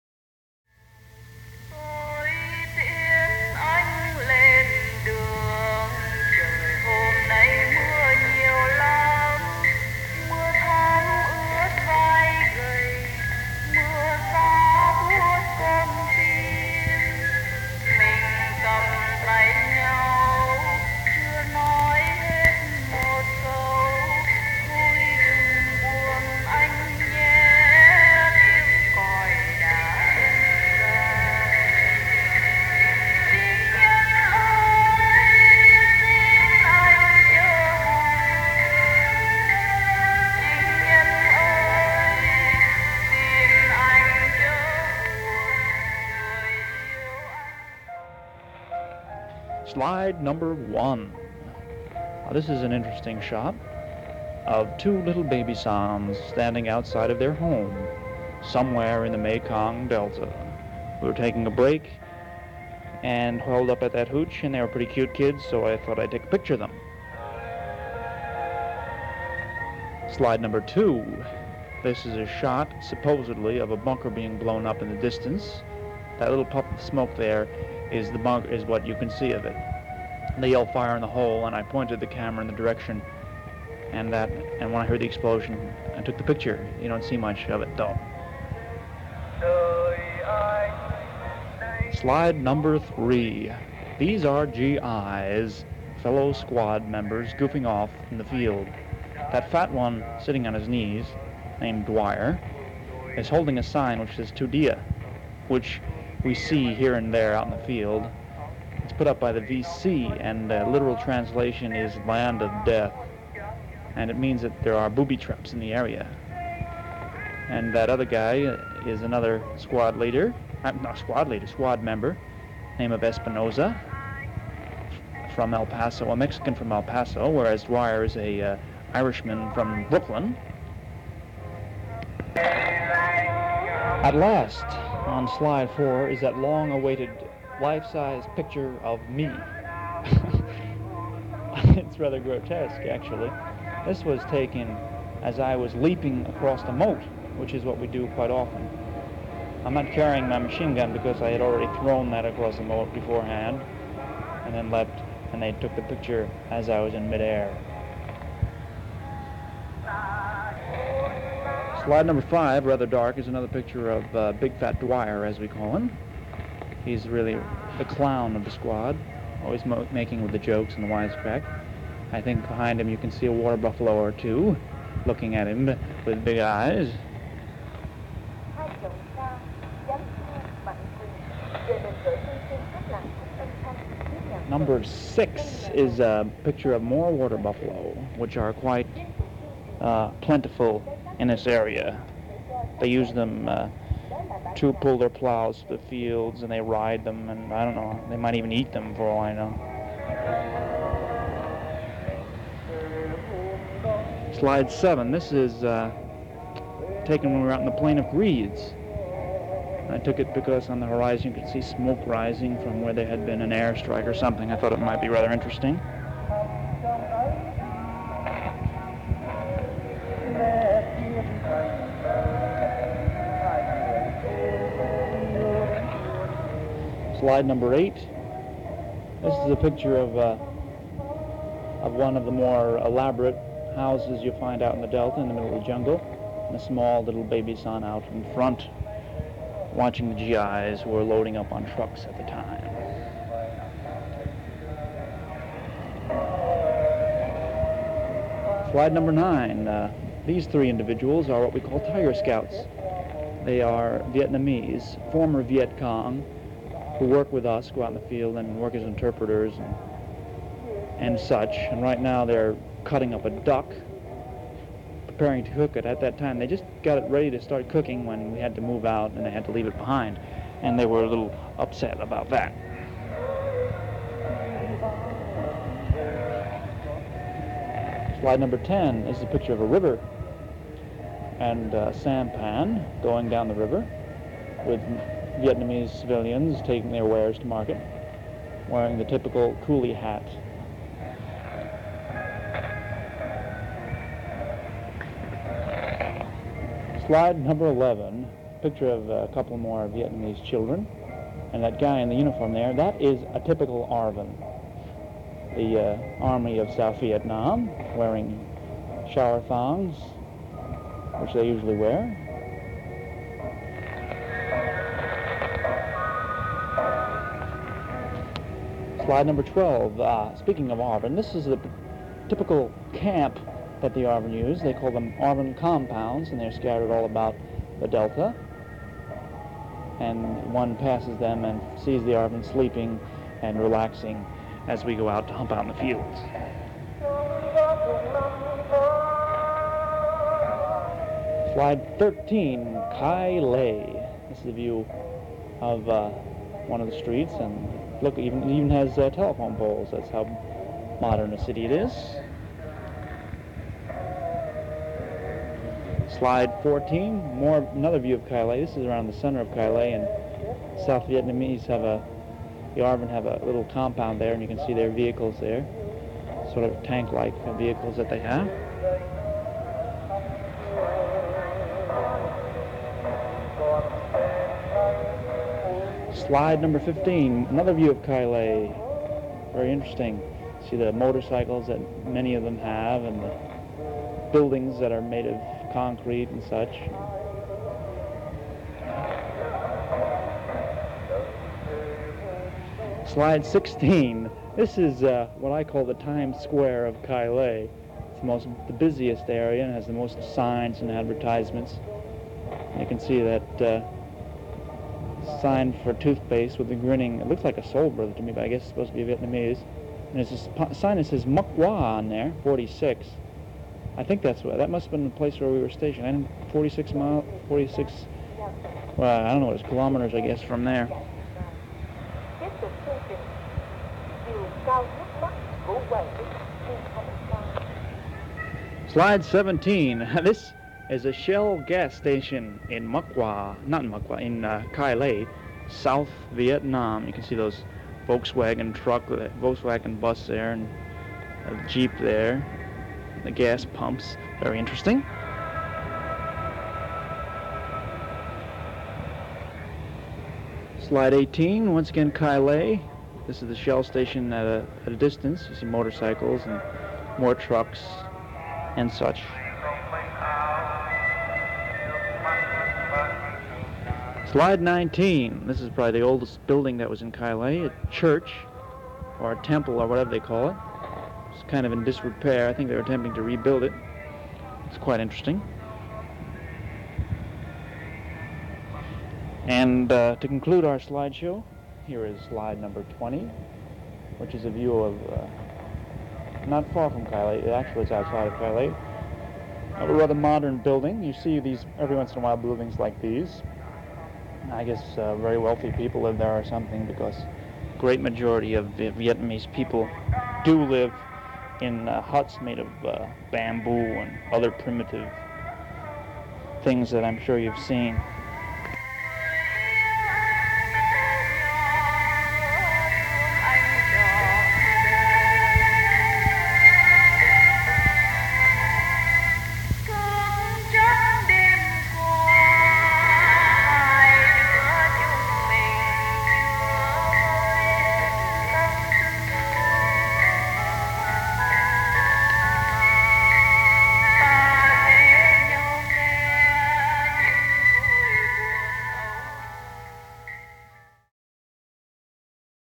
Vietnam Slideshow 2
The second of 2 slideshows I made in Vietnam in 1969. The narration recorded in Dong Tam is sometimes not the best audio quality. I have many other photos that will be made into another video eventually. There is also a Flash presentation of more of my Vietnam experiences